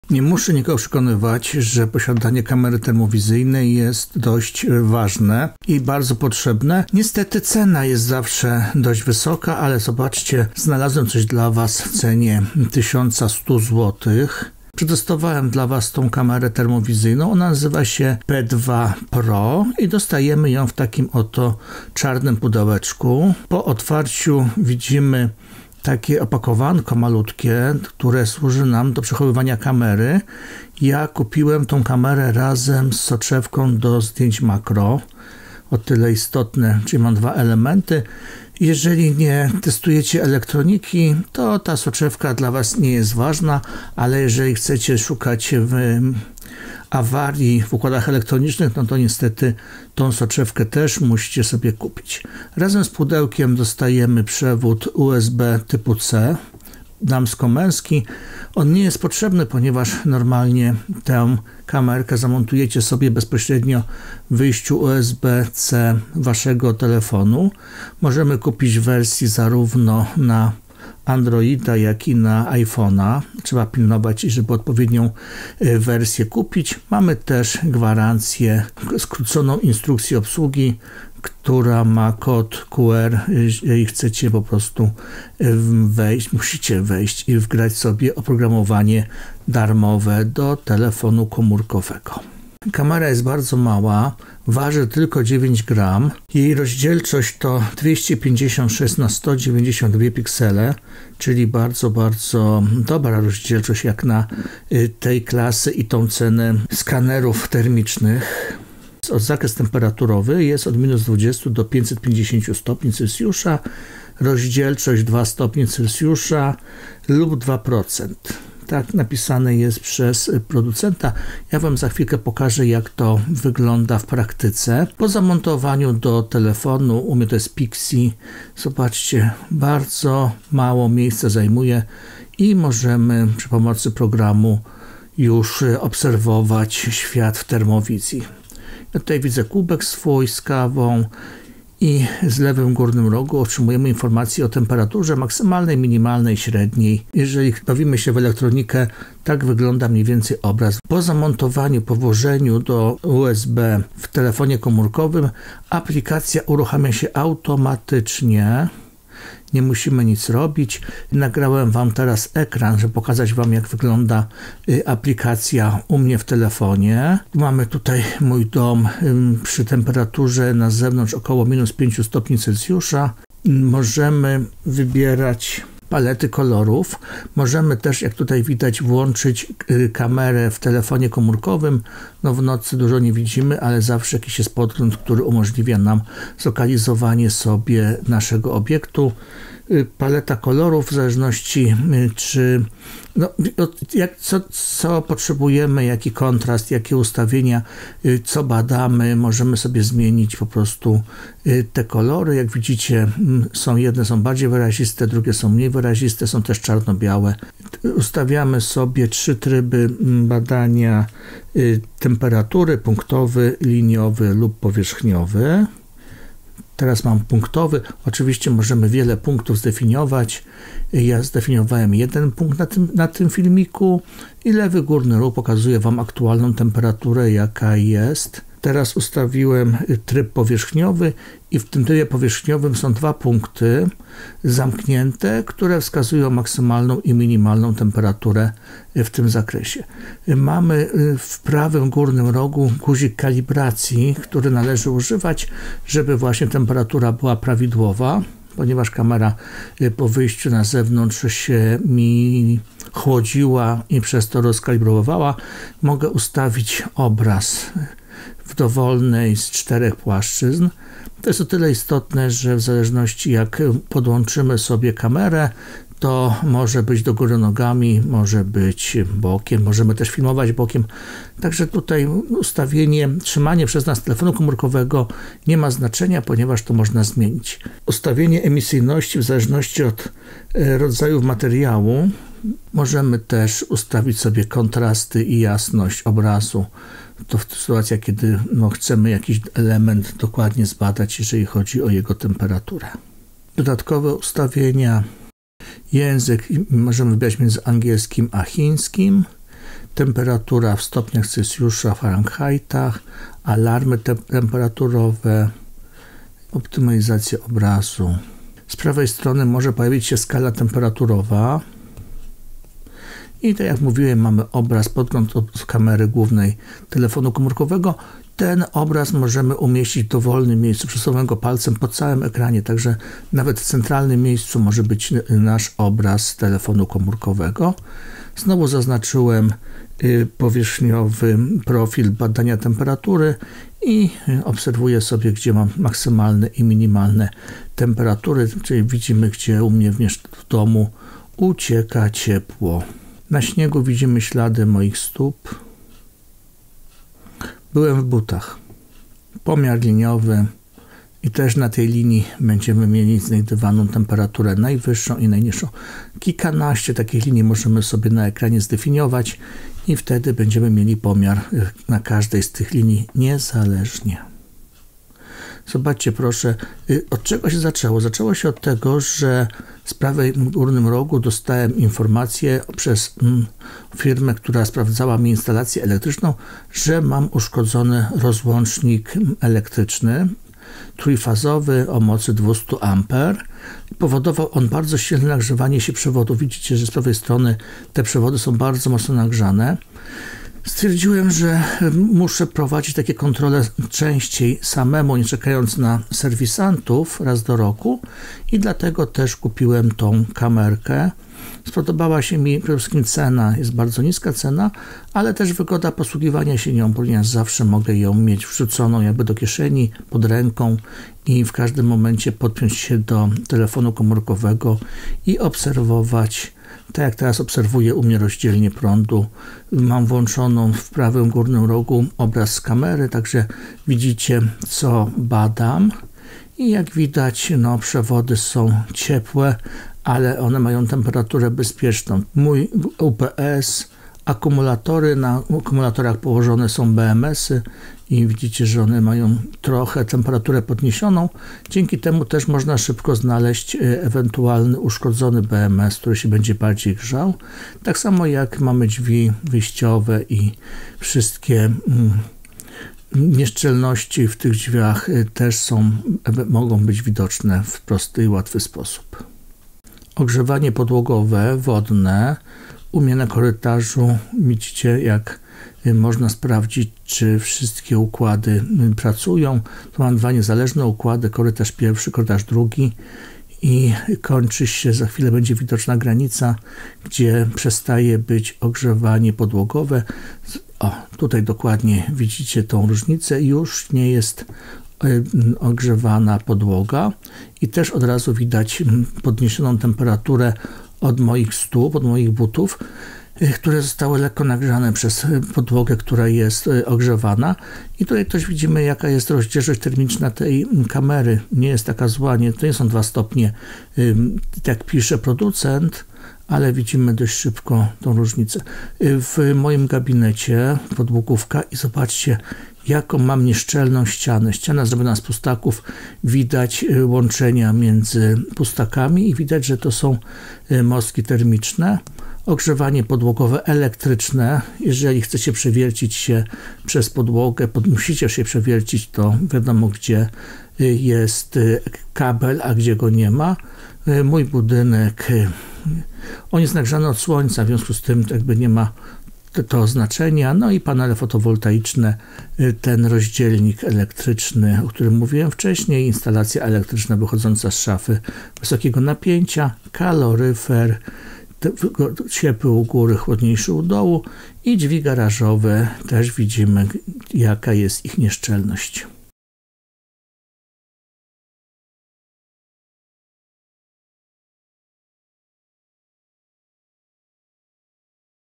INFIRAY P2 Pro dobrej jakości kamera termowizyjna USB - prezentacja
INFIRAY P2 Pro kamera termowizyjna
*Android | USB-C | 256 x 192 px | Obiektyw makro*
10% zniżki na hasło: P2PINFI3
Pamiętaj o zniżkowym kodzie - P2PINFI3

*jakość obrazu*
Sensor kamery rejestruje obraz w rozdzielczości aż 256 x 192 pikseli - spotykanej zwykle w zaawansowanych, kilkukrotnie droższych urządzeniach.
*Picture-in-picture*
Kamera umożliwia wyświetlanie obrazu ze zwykłej kamery smartfona nałożonego na podgląd termowizyjny
*Kamera nie potrzebuje baterii ani ładowarek*. Jest zasilana bezpośrednio przez telefon.
Do kamery dołączony jest specjalny obiektyw umożliwiający obserwowanie temperatury bardzo małych elementów w np. układach elektronicznych.